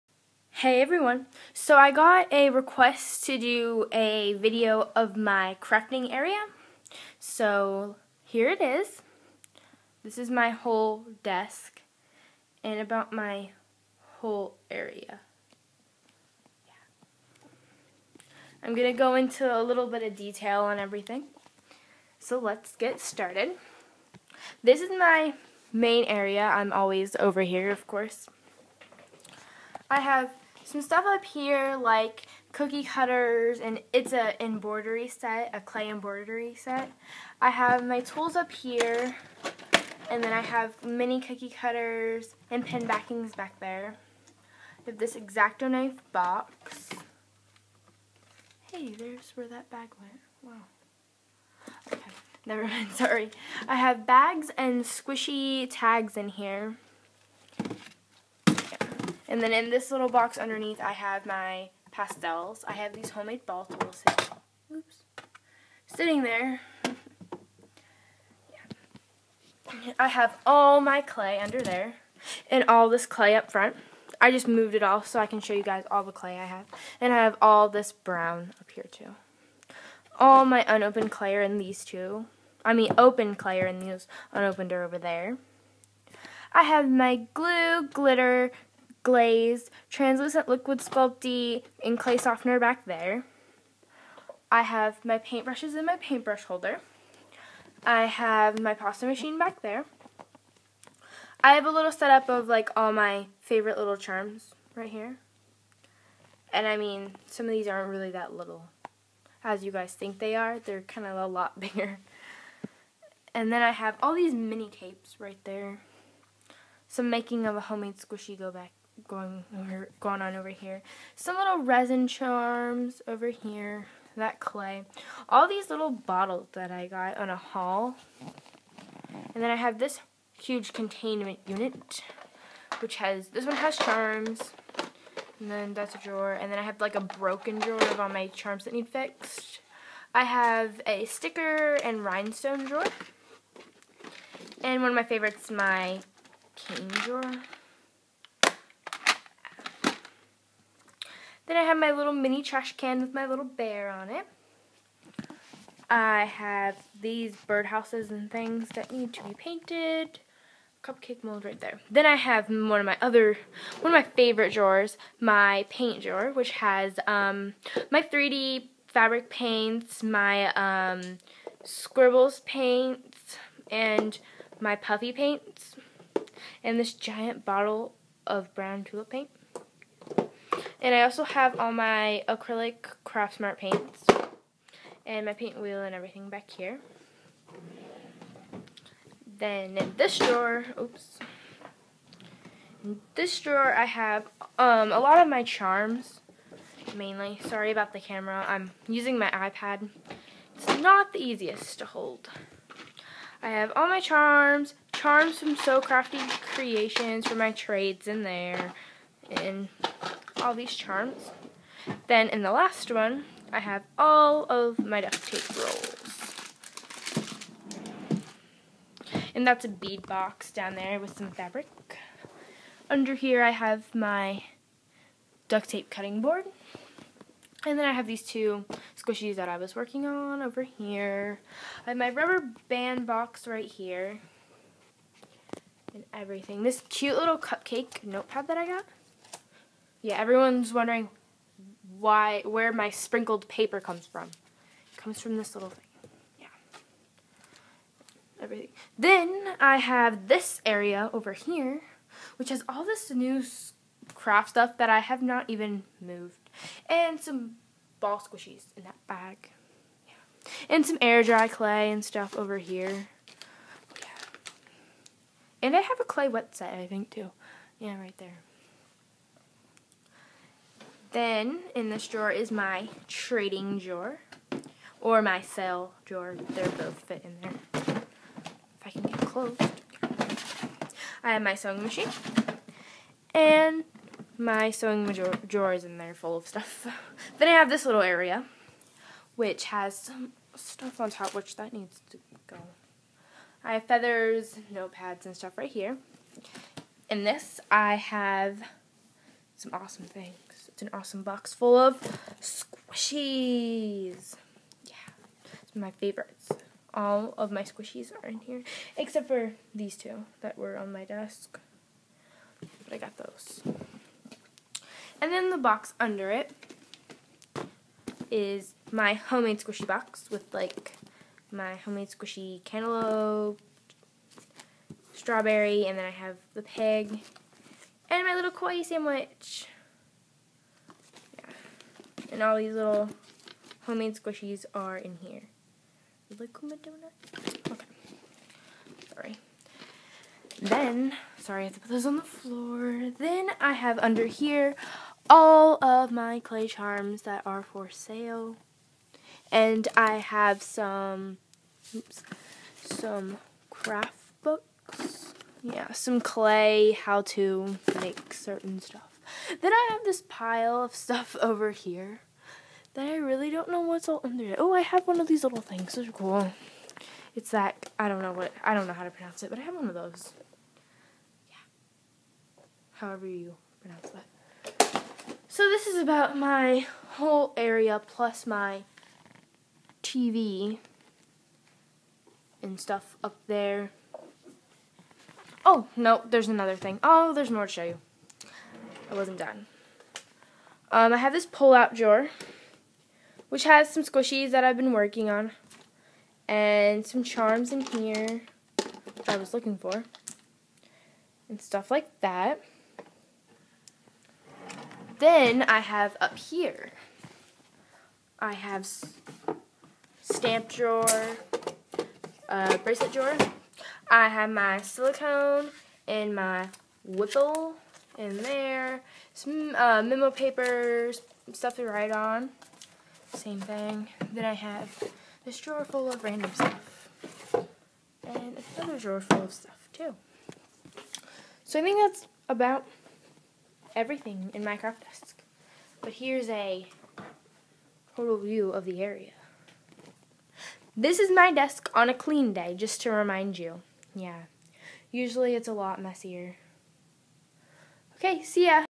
My craft area
Just a video of my craft desk and everything.